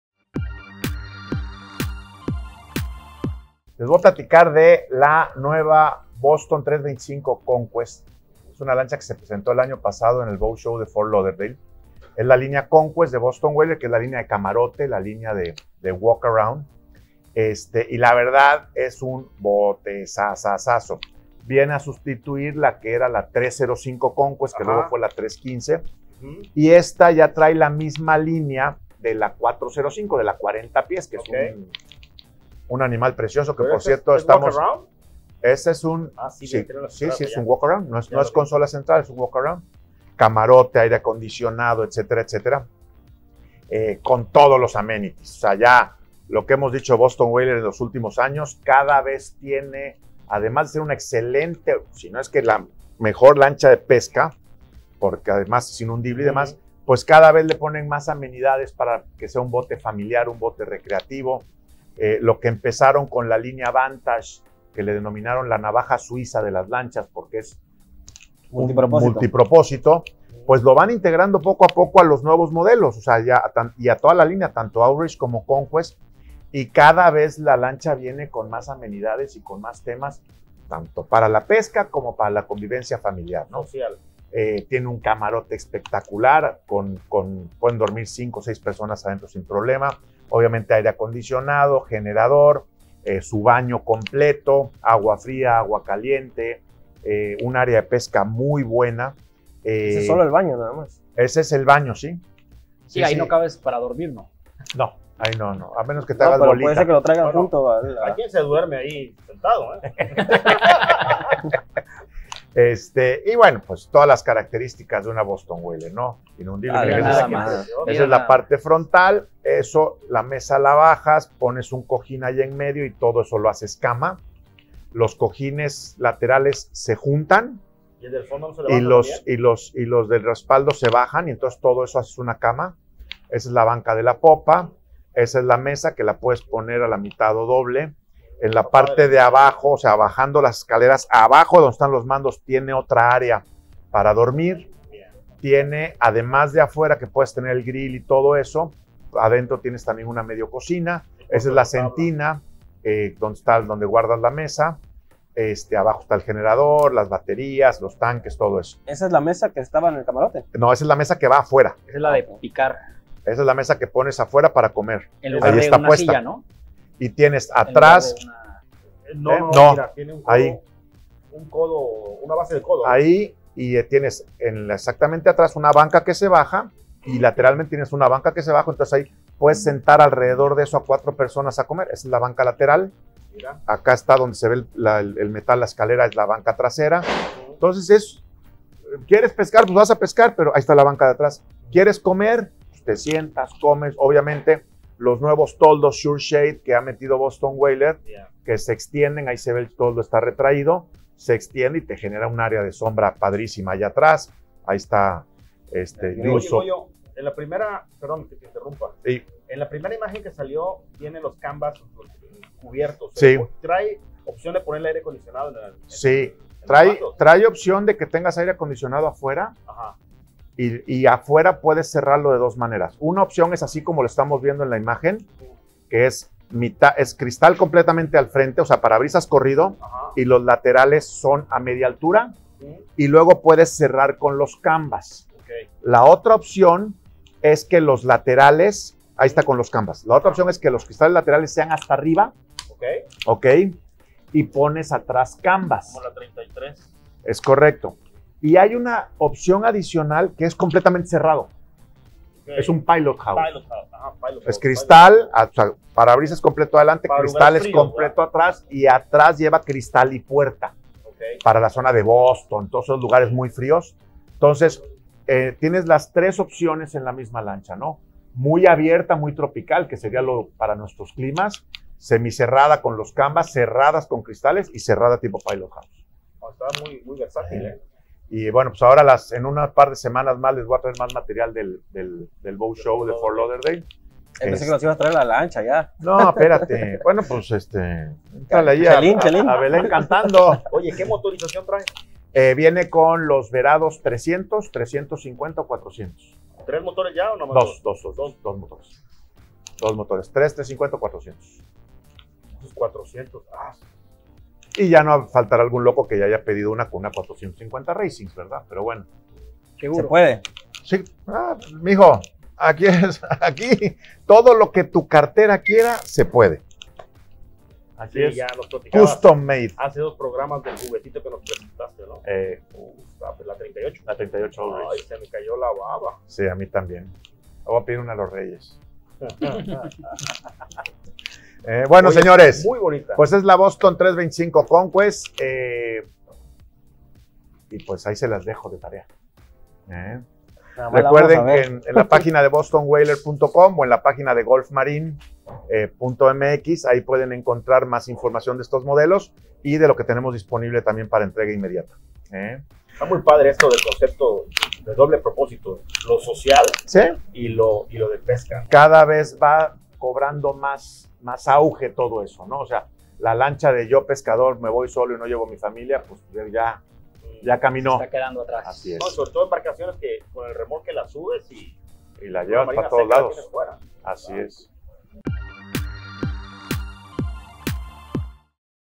Nueva Boston 325 Conquest
En esta cápsula Goyo nos presenta a la nueva Boston 325 Conquest.